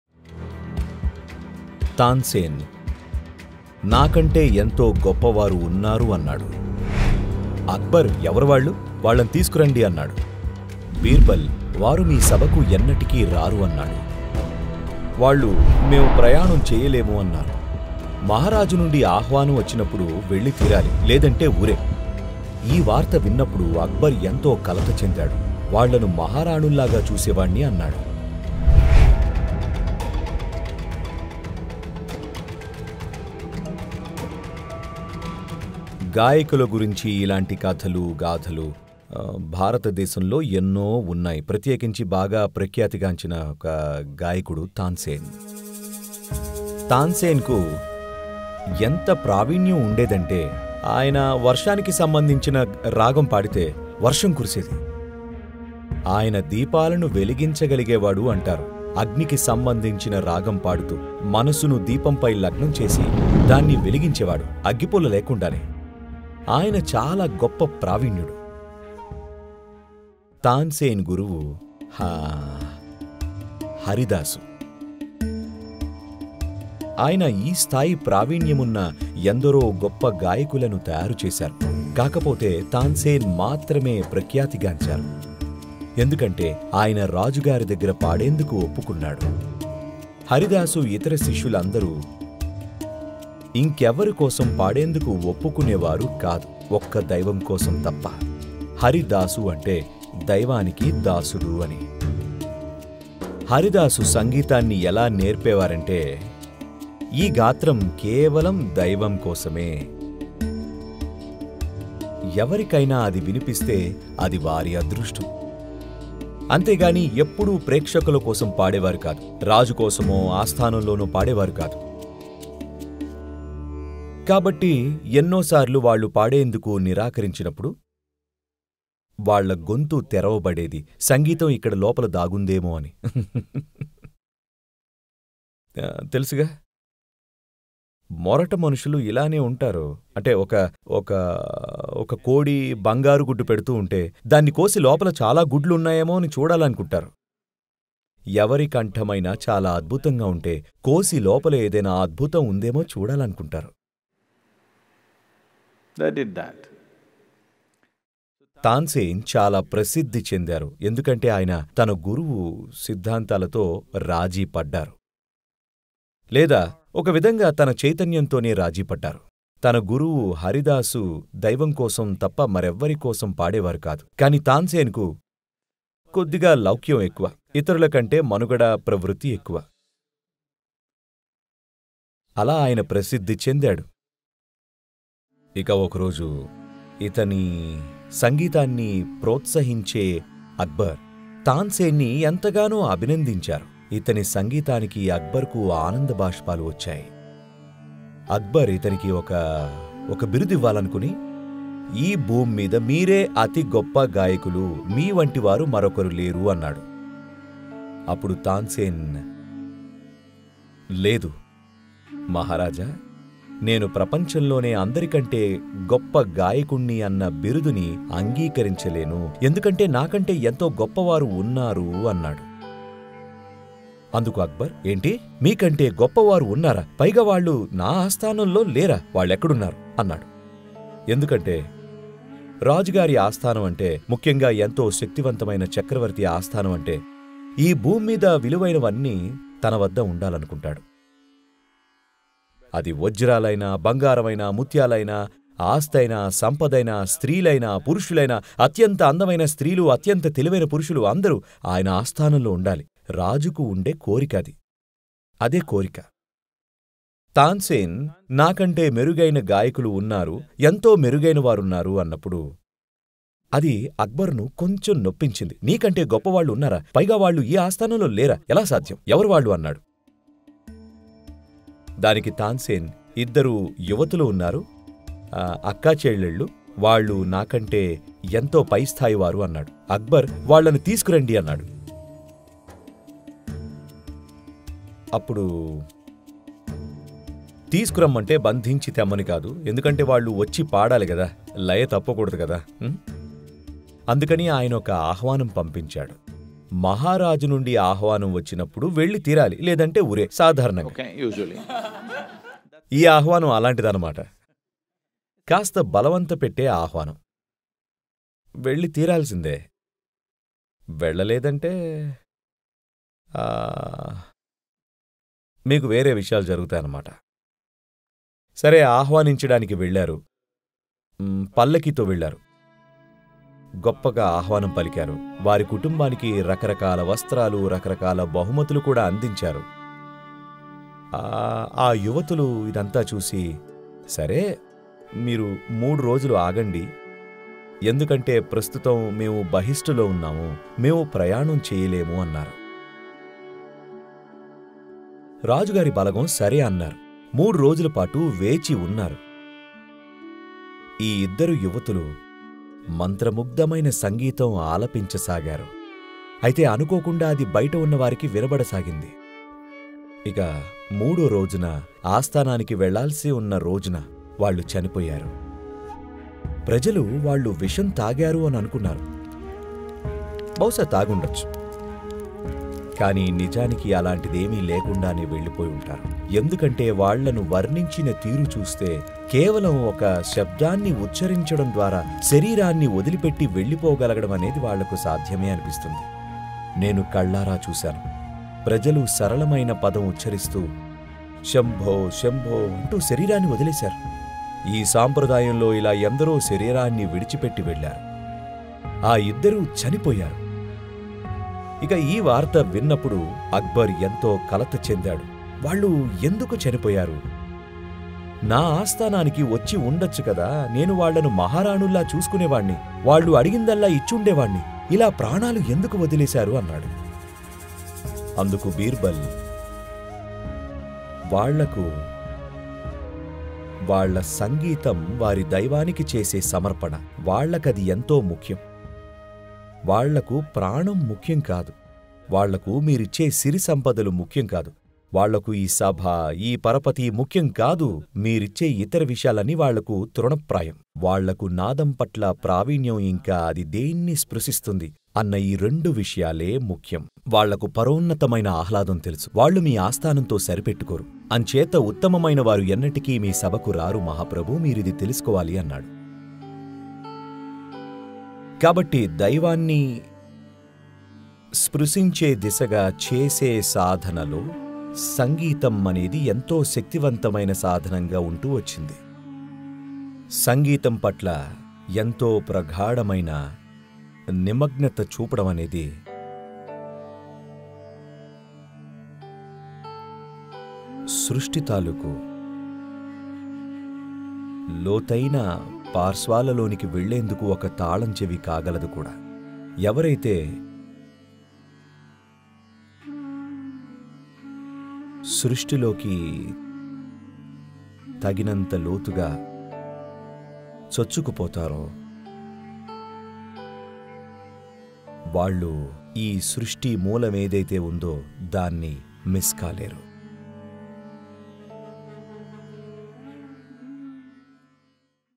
అక్బర్, బీర్బల్ & తాన్సేన్ గురించి ఒక ఆసక్తికరమైన కథ An Untold Story About Akbar Birbal And Tansen
సద్గురు,  అక్బర్ ఆస్థానంలోని  సంగీత గాయకుల్లో శిఖామణి అయిన తాన్సేన్ గురించి, ఇంకా అక్బర్ ఆస్థానంలో పాడవలసిందిగా  వచ్చిన ఆహ్వానాన్ని తిరస్కరించిన ఉద్దండులైన ఇద్దరు మహిళా గాయకులను గురించిన కథను వివరిస్తున్నారు. ఆహ్వానాన్ని మన్నించే బదులు వారు తమ ప్రాణాలనే త్యజించారు. అలాగే ఆయన  సృష్టి మూలాన్ని స్పృశించేందుకు సంగీతాన్ని ఇంకా శబ్దాన్ని వినియోగించడం గురించి కూడా మాట్లాడారు.